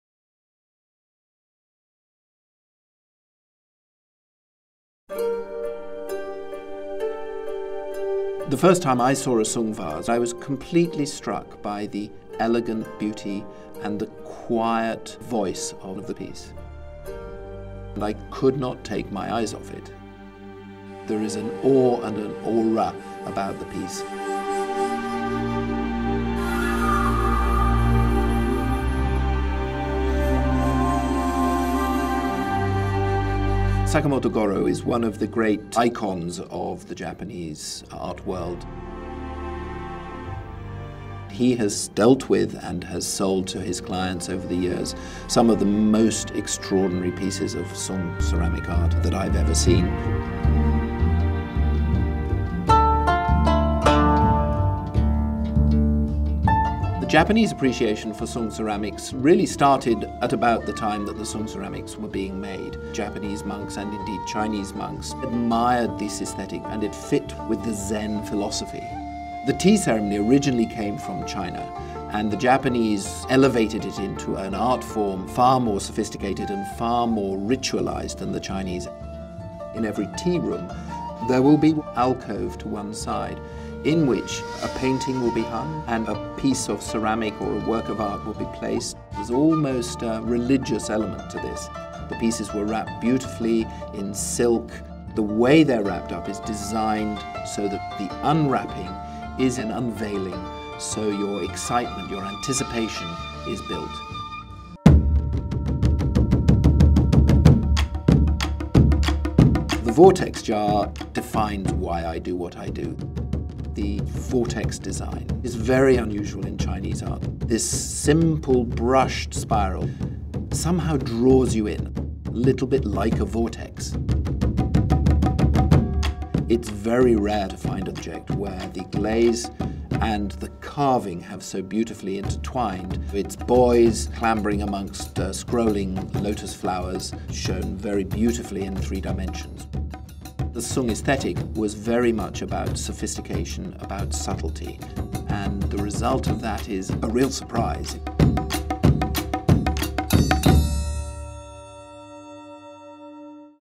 The Reverence of Song Ceramics: Sakamoto Goro
Join Henry Howard-Sneyd as he reveals the elegant beauty of Song ceramics. Learn more about Sakamoto Goro, an icon of the Japanese art world who collected some of the most extraordinary pieces of Song ceramic art, and discover how the Japanese appreciation for Song ceramics has evolved into an art form.